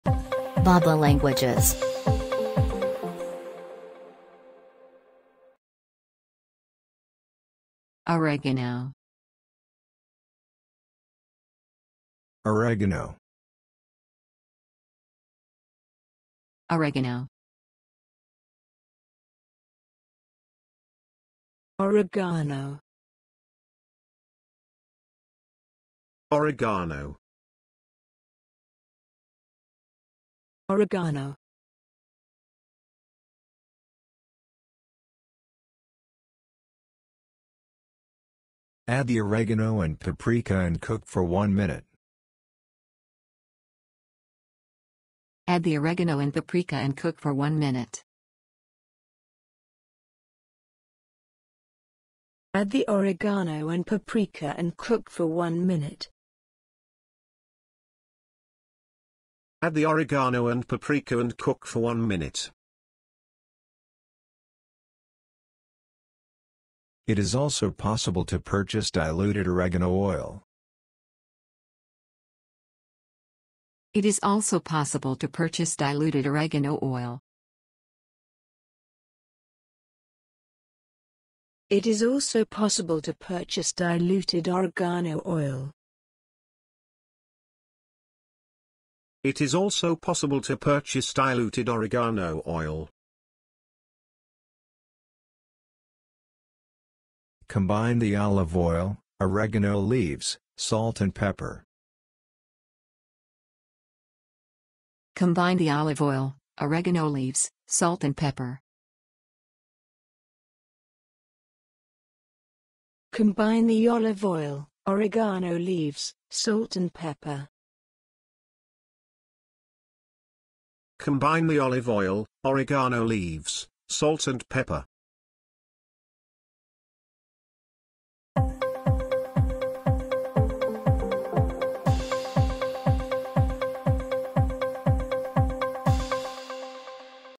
OREGANO pronunciation | Improve your language with bab.la
Improve your spoken English by listening to OREGANO pronounced by different speakers – and in example sentences too. Learn and love languages with bab.la.